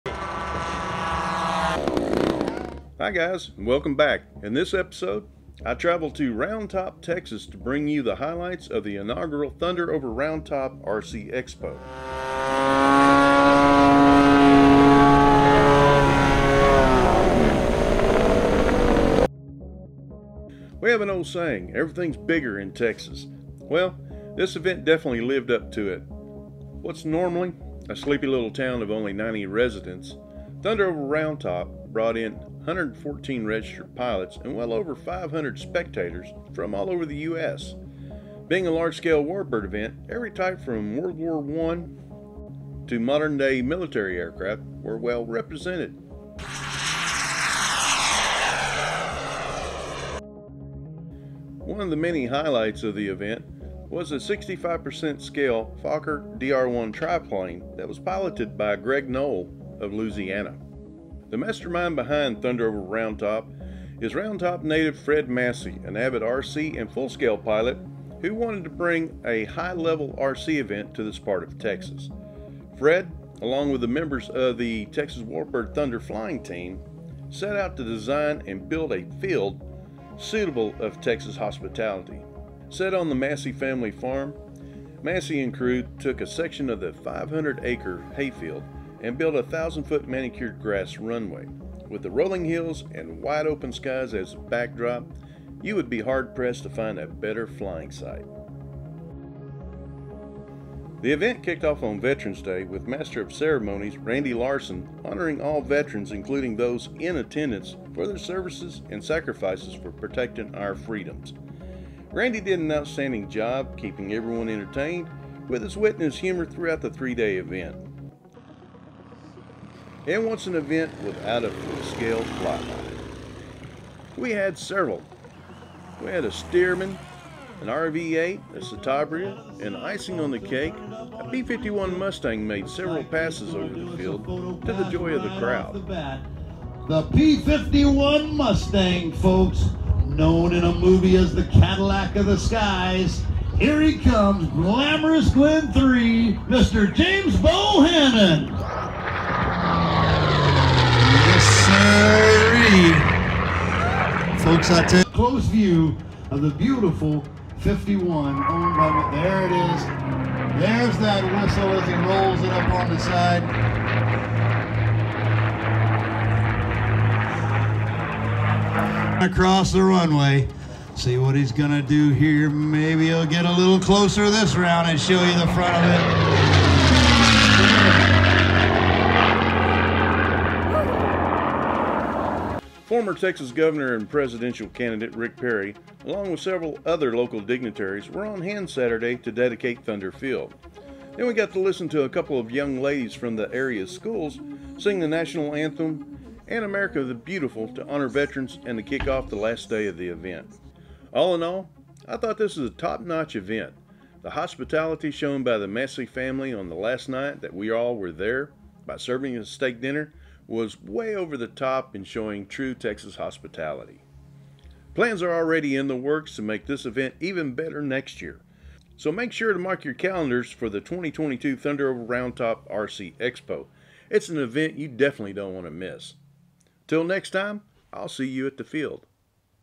Thunder Over Round Top RC Expo
In this episode, I travel to Round Top Texas for the inaugural Thunder Over Round Top RC Expo. This was the best event to end the year with! 114 pilots, over 500 spectators and more Texas hospitality than you can shake a stick at! Here are the highlights of the event. Be sure to add Thunder Over Round Top 2022 on your calendar of events to make. You Won't be disappointed!!!

Want to become an RC pilot? Check out this video to get started in this time honored hobby

Get started with the right gear.
Flight Simulator